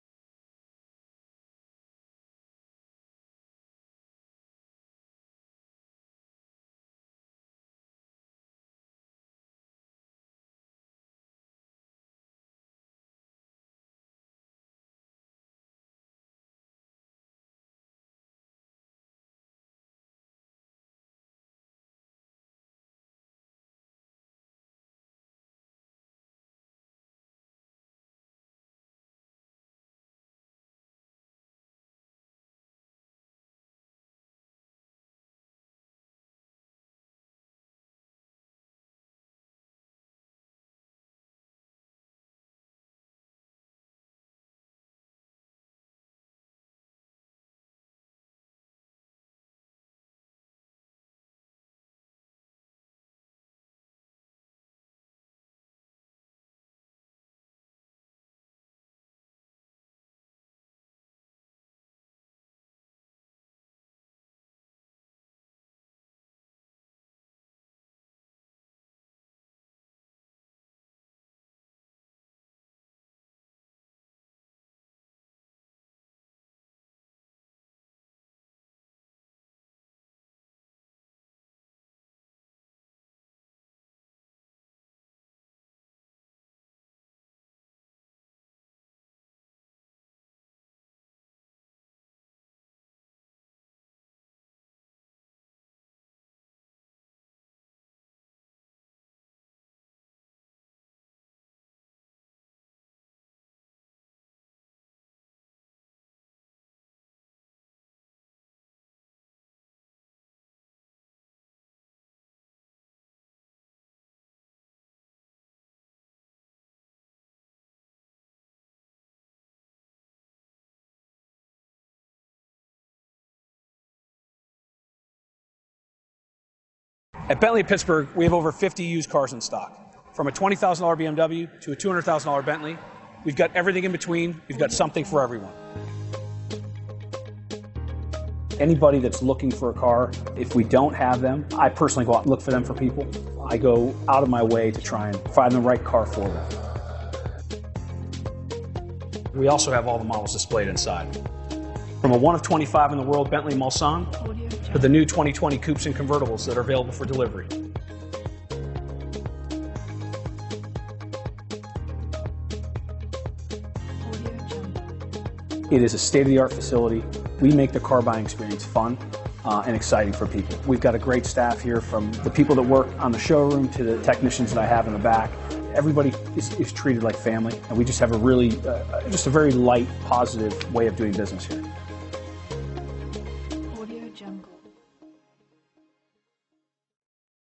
2019 Maserati Levante Pittsburgh, Wexford, Cranberry Township, PA M19046
Nero Noctis Black New 2019 Maserati Levante available in Wexford, Pennsylvania at Rohrich European Motors Bentley Maserati. Servicing the Pittsburgh, Cranberry Township, PA area.

Rohrich European Motors Bentley Maserati
15001 Perry Hwy

Nero Noctis Black 2019 Maserati Levante AWD ZF 8-Speed Automatic 3.0L V6 ABS brakes, Compass, Electronic Stability Control, Front dual zone A/C, Heated door mirrors, Heated front seats, Illuminated entry, Leather Upholstery, Low tire pressure warning, Navigation System, Power Liftgate, Remote keyless entry, Traction control.